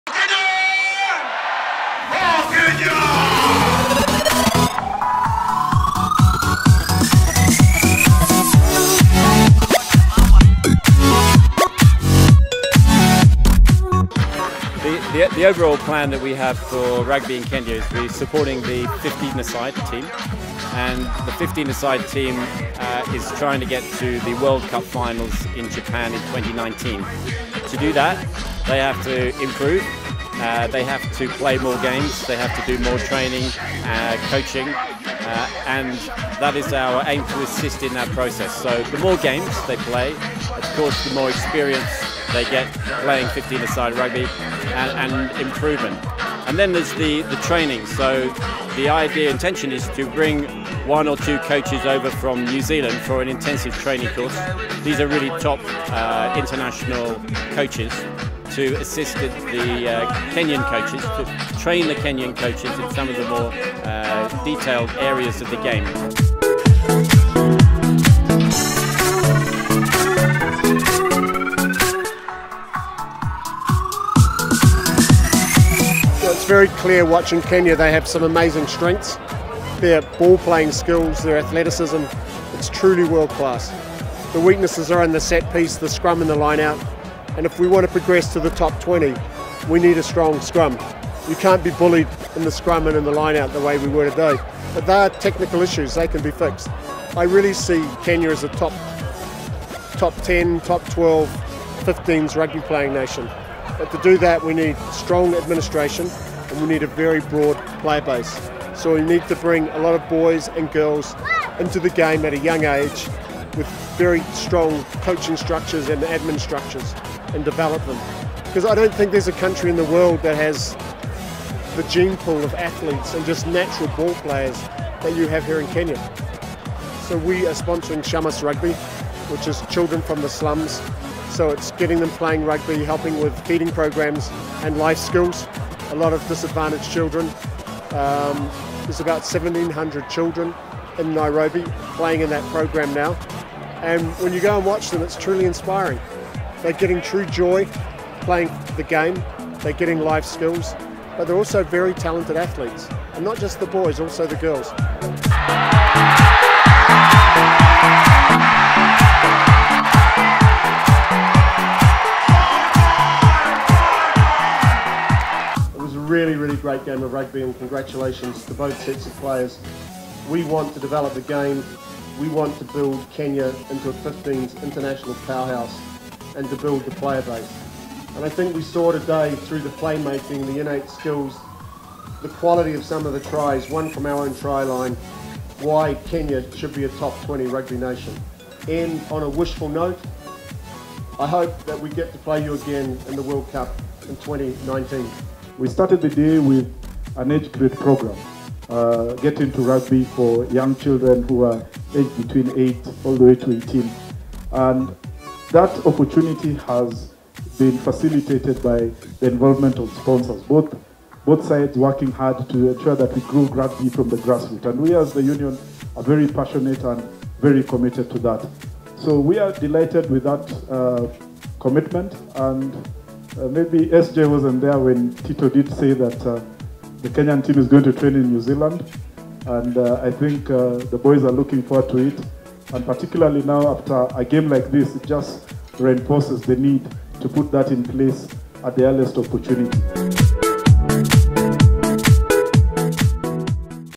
Tatu City Series Kenya Vs Germany Rugby 15s Highlights May 2017
A great day of rugby at the Tatu City Series which saw the Kenya Simbas lose narrowly to Germany in a very packed RFUEA Grounds. We thank you all for supporting the game and for supporting our team.